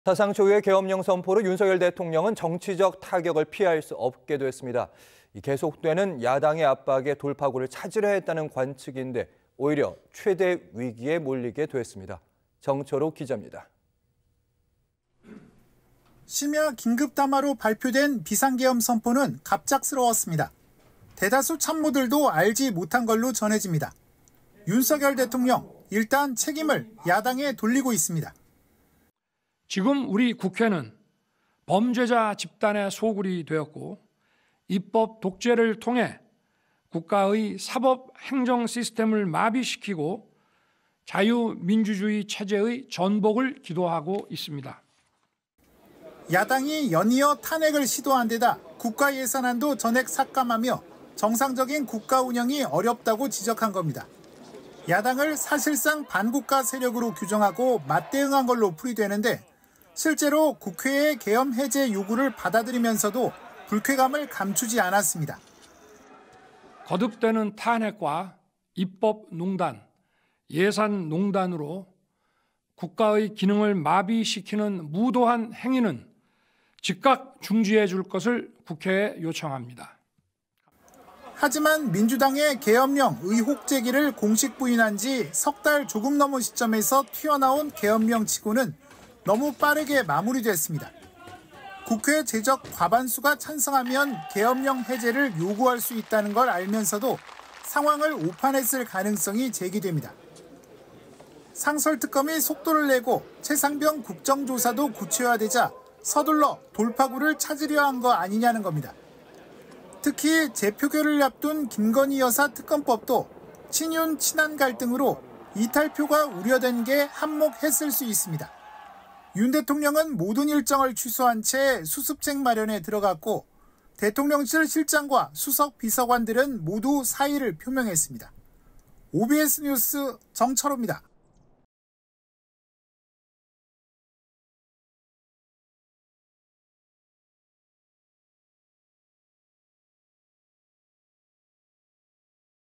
윤 대통령은 왜 계엄을 택했나? 야당 압박 돌파구 찾으려다 최대 위기
【앵커】
사상 초유의 계엄령 선포로
윤석열 대통령, 정치적 타격을
피할 수 없게 됐습니다.
계속되는 야당의 압박에
돌파구를 찾으려 했다는 관측인데,
오히려 최대 위기에 몰리게 됐습니다.
정철호 기자입니다.

【기자】

심야 긴급 담화로 발표된 비상 계엄 선포는 갑작스러웠습니다.

대다수 참모들도 알지 못한 걸로 전해집니다.

윤석열 대통령, 일단 책임을 야당에 돌리고 있습니다.

[윤석열 / 대통령: 지금 우리 국회는 범죄자 집단의 소굴이 되었고 입법 독재를 통해 국가의 사법행정 시스템을 마비시키고 자유민주주의 체제의 전복을 기도하고 있습니다.]

야당이 연이어 탄핵을 시도한데다 국가 예산안도 전액 삭감하며 정상적인 국가 운영이 어렵다고 지적한 겁니다.

야당을 사실상 반국가세력으로 규정하고 맞대응한 걸로 풀이되는데, 실제로 국회의 계엄 해제 요구를 받아들이면서도 불쾌감을 감추지 않았습니다.

[윤석열 / 대통령: 거듭되는 탄핵과 입법 농단, 예산 농단으로 국가의 기능을 마비시키는 무도한 행위는 즉각 중지해 줄 것을 국회에 요청합니다.]

하지만 민주당의 계엄령 의혹 제기를 공식 부인한지 석 달 조금 넘은 시점에서 튀어나온 계엄령 치고는 너무 빠르게 마무리됐습니다.

국회 재적 과반수가 찬성하면 계엄령 해제를 요구할 수 있다는 걸 알면서도 상황을 오판했을 가능성이 제기됩니다.

상설특검이 속도를 내고 채상병 국정조사도 구체화되자 서둘러 돌파구를 찾으려 한 것 아니냐는 겁니다.

특히 재표결을 앞둔 김건희 여사 특검법도 친윤, 친한 갈등으로 이탈표가 우려된 것이 한 몫 했을 수 있습니다.

윤 대통령은 모든 일정을 취소한 채 수습책 마련에 들어갔고 대통령실 실장과 수석 비서관들은 모두 사의를 표명했습니다.

OBS뉴스 정철호입니다.